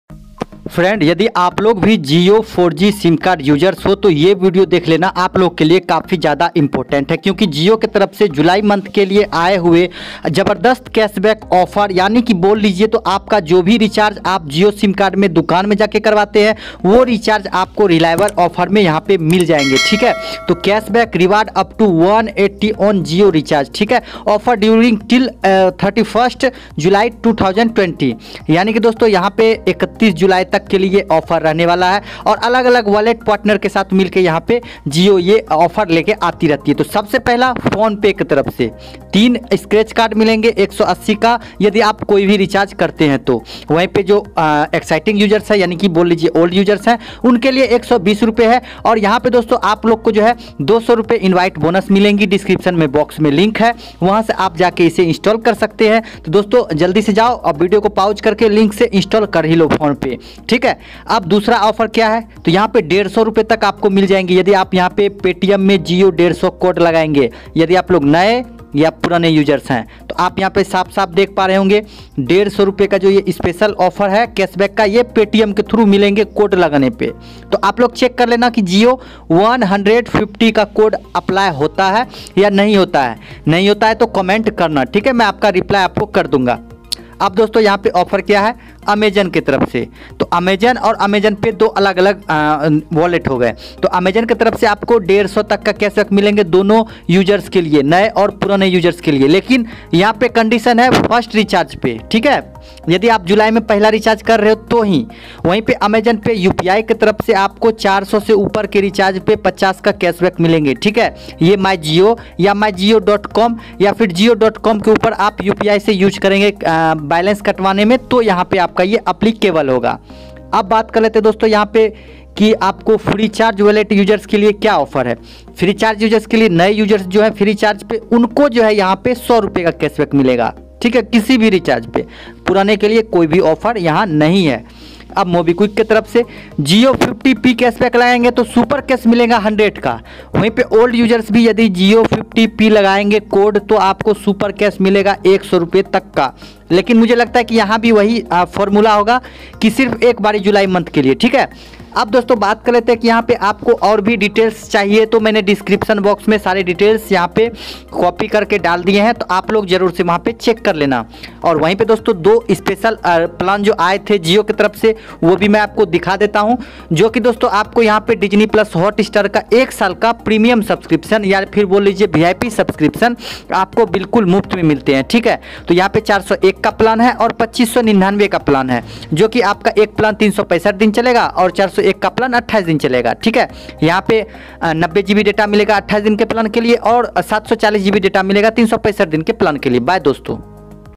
बड़ी खुशख़बरी | JIO RECHARGE ₹180 CASHBACK OFFERS | Jio OLD Plan ₹149,₹399 Are Eligible | July Offer
Wallet Cashback applicable on recharge through partner App or Site on minimum/above recharge value. Please refer to the partner's site or app for complete Offer T&Cs
PhonePe Offer: First Jio recharge has to be Rs.125 & above in the month of July to be eligible for the offer
Freecharge Offer: User clicking on the linking will see for the recharge value the applicable code on the page